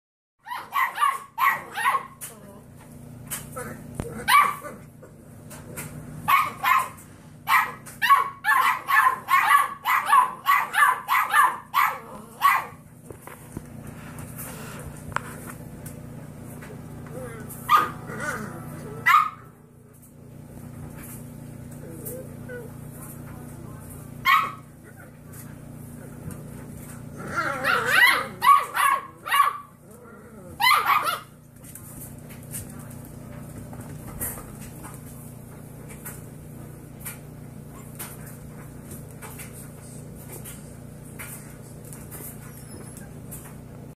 Puppy Bark Off
You've got to love the learning to bark phase of raising puppies.  It's just adorable.  Foster puppies having a bark off.  They're just started really barking the last few weeks.  6 wks old